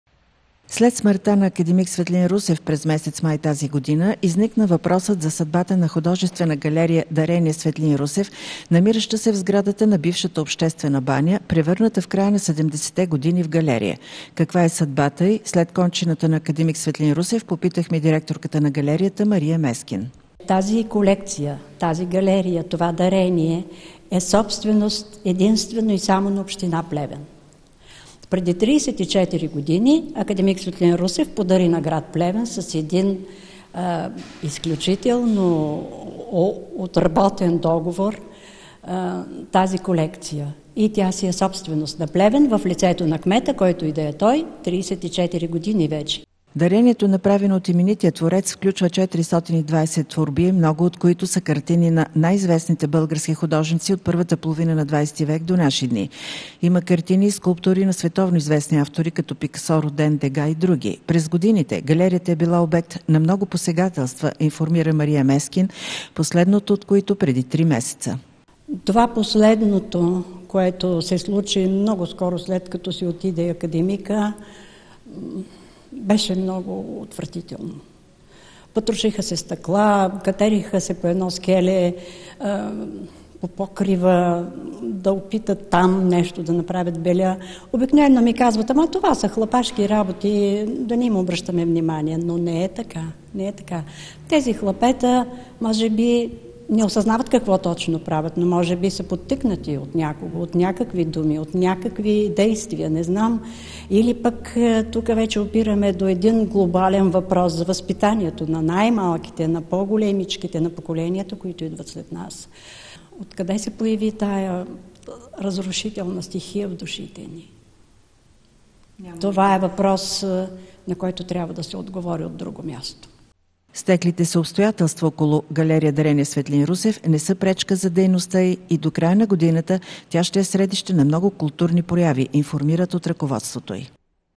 Художествена галерия “Дарение Светлин Русев“ остава собственост на община Плевен
Художествена галерия „Дарение Светлин Русев“ остава собственост на община Плевен и след смъртта на академик Светлин Русев. Това заяви директорката на галерията Мария Мескин